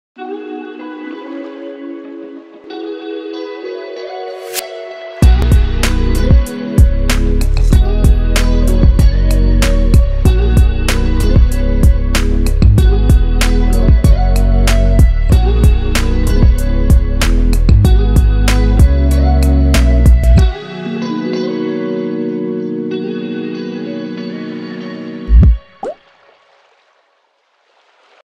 Storage Cabinet with Sliding Barn Door, Home Furniture, Home Improvement - VASAGLE - ULSC88BX
Industrial design storage cabinet with sliding barn door for kitchen, bedroom, living room, and home office - ULSC88BX here from VASAGLE. Whether you’re looking for a living room cabinet for movies and games, a kitchen cabinet for plates and bowls, or an extra storage solution in the bedroom, this industrial floor cabinet is ready to give your home a brilliant upgrade.